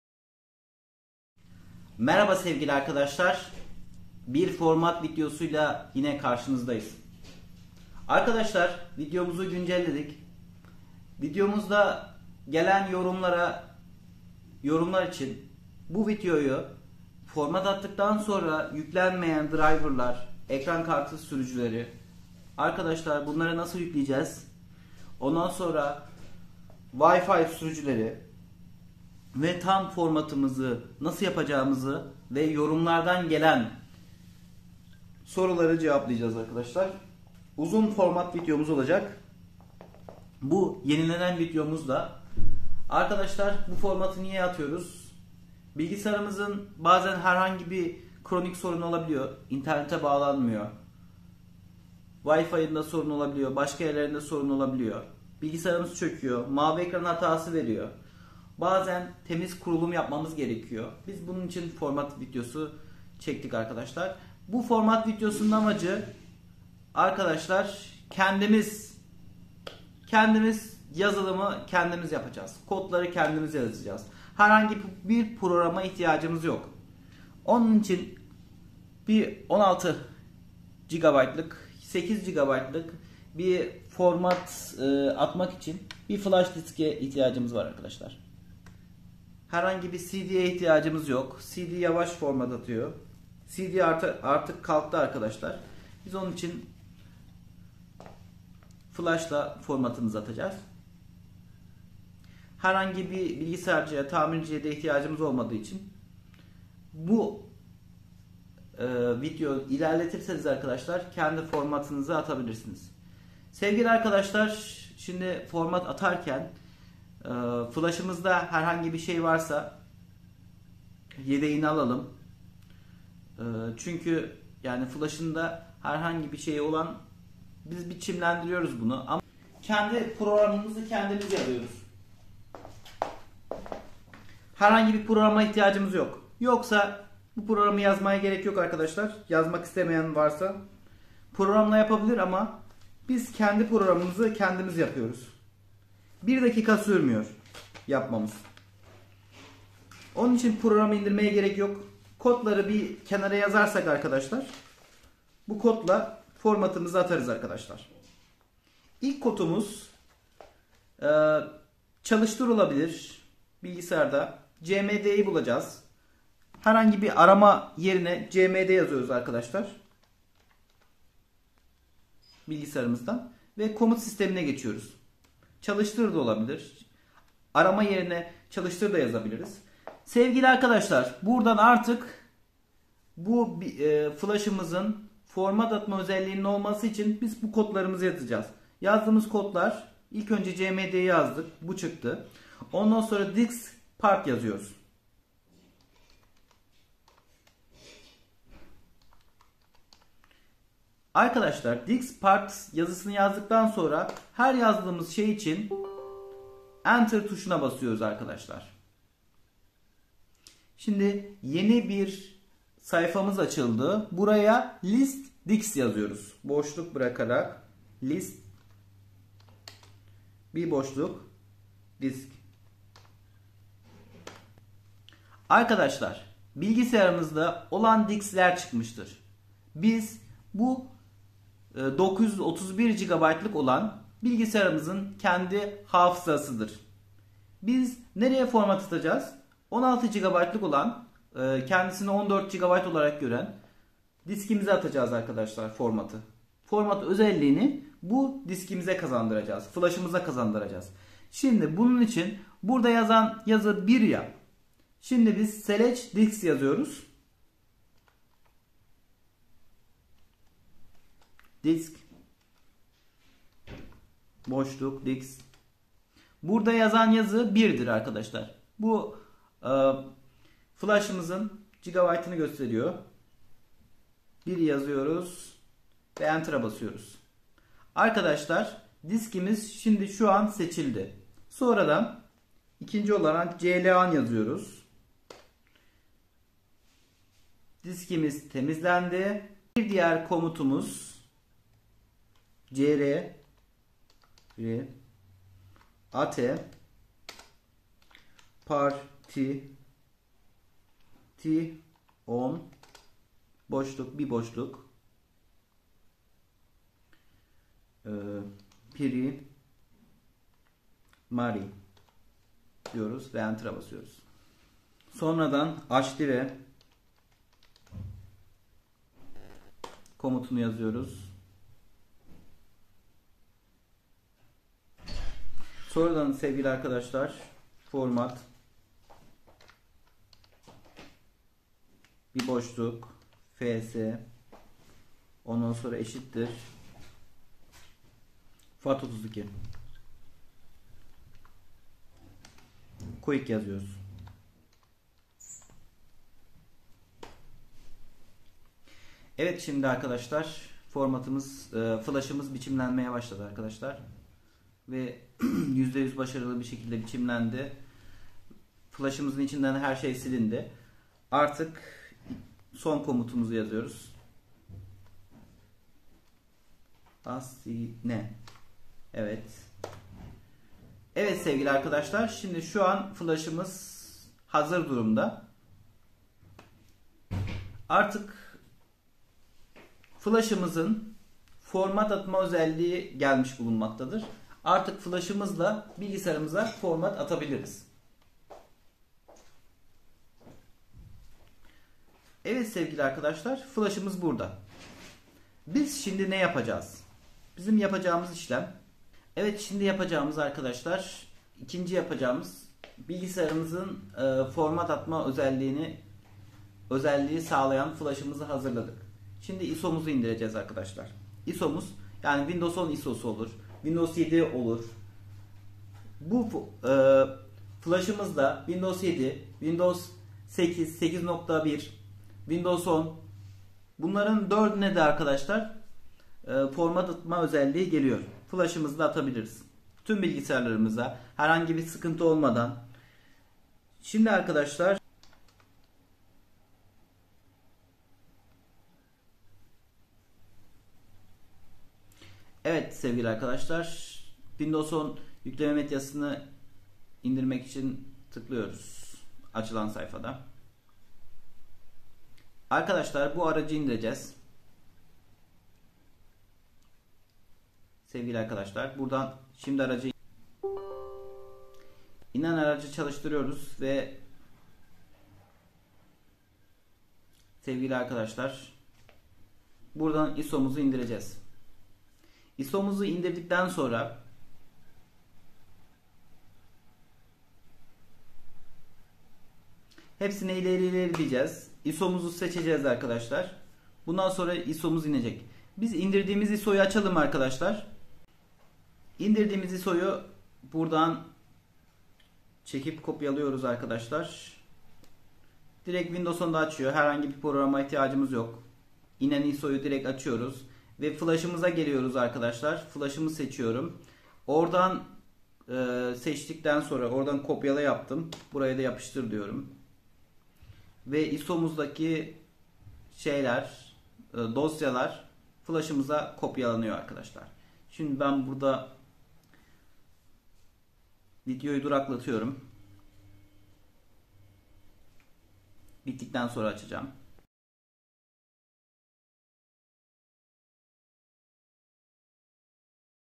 Usb Flash Bellek Format Atma (Programsız) Yenilendi ve full Part 1
usb flash bellek format atma
Windows 10 usb flash bellek format atma (programsız)
windows 8.1 , windows 8  windows 7 usb flash bellek format atma programsız
  çalıştır  aç : cmd kodlar:diskpart,list disk,select disk 2,clean,create partition primary,active,format fs=fat32 quick,assign ve her defasında enter